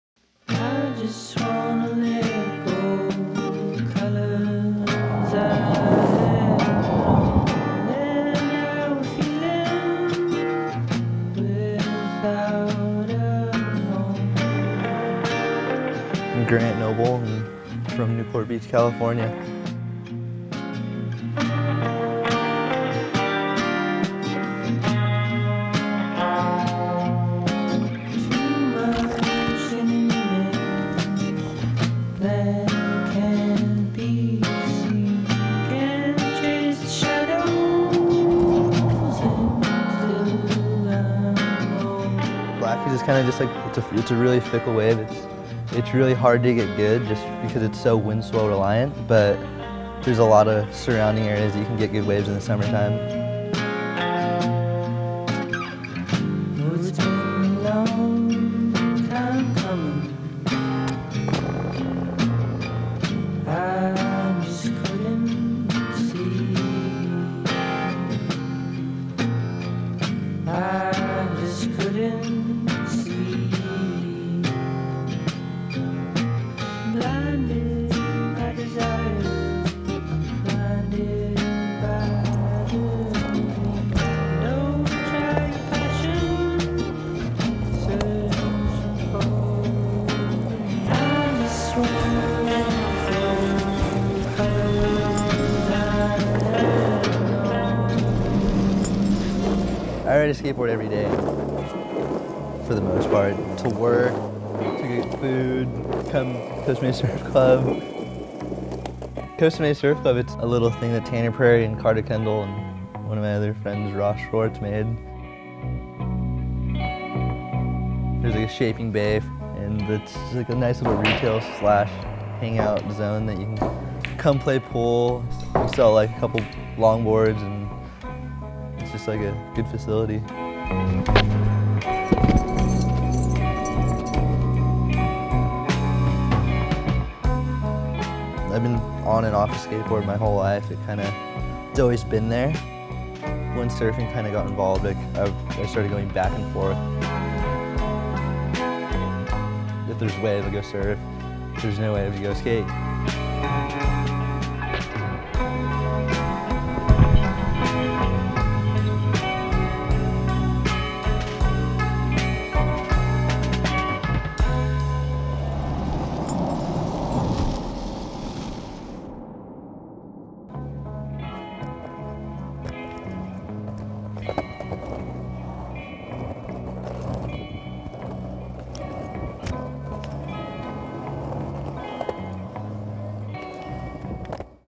Penny’s OPEN ROADS Series – Episode 02: Grant Noble
Our 'Open Roads' series is dedicated to chronicling the everyday adventures of creative individuals from all walks of life. Grant's a multi-talented Newport Beach surfer, skateboarder and photographer. This episode follows Grant, his girlfriend Emma and a few of his friends as they surf and skate in Newport Beach and hangout at the Costa Mesa Surf Club.

Learn more about Grant and see behind the scenes photos

Filmed and Edited by George Trimm: buzz-cut.com

Check out Grant’s Instagram to find out what he’s up

Find out more about the Costa Mesa Surf Club: costamesasurfclub.com